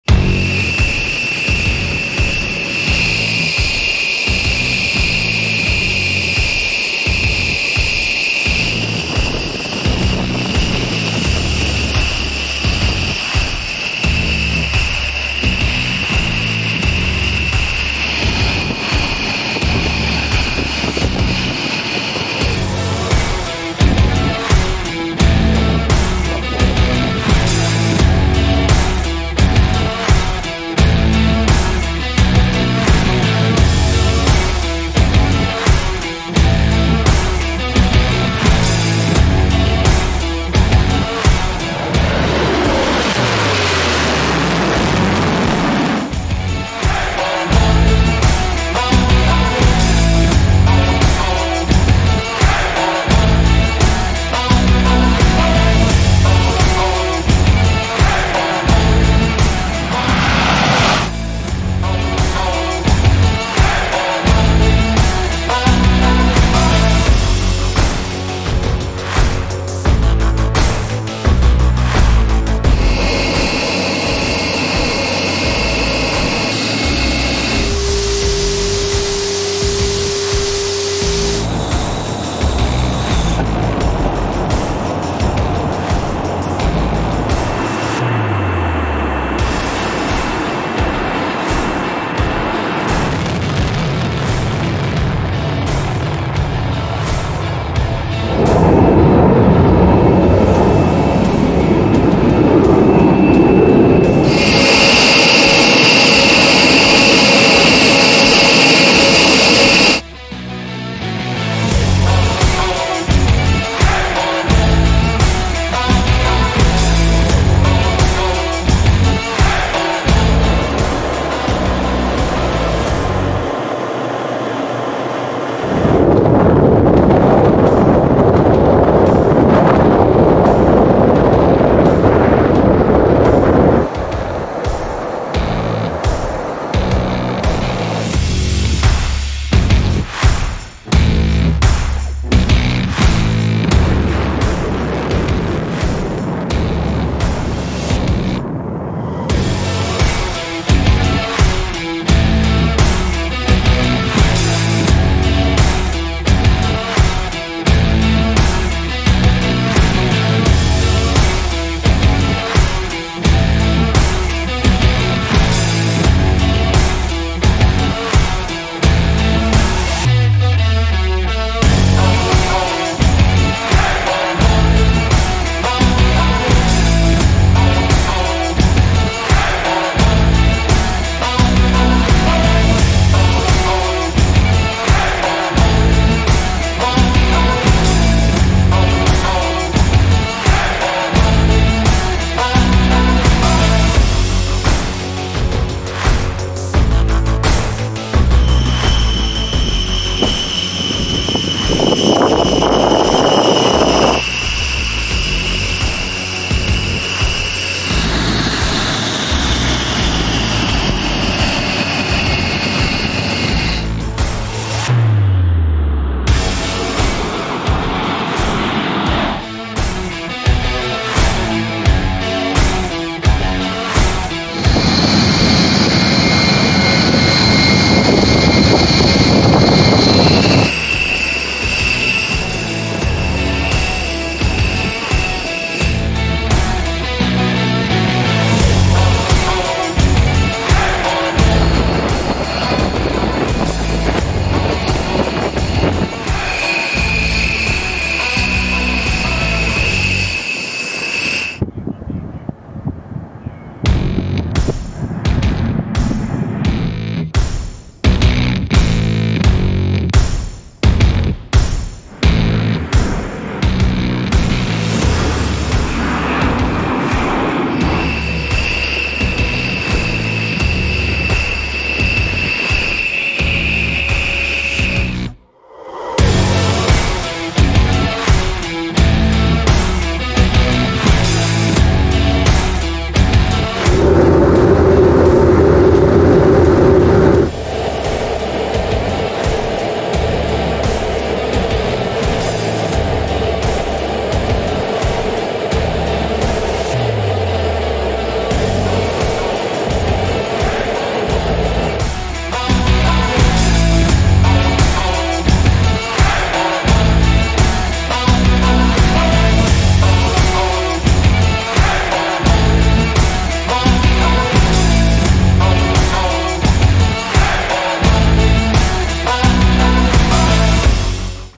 B-1B Lancer, Unleashing the ‘Bone’
Get ready for an adrenaline-fueled ride with the B-1B Lancer! Nicknamed the ‘Bone,’ this supersonic bomber combines speed, power, and precision. From its fixed intake ramps to its impressive payload, we delve into what makes the B-1B a force to be reckoned with! 🚀💥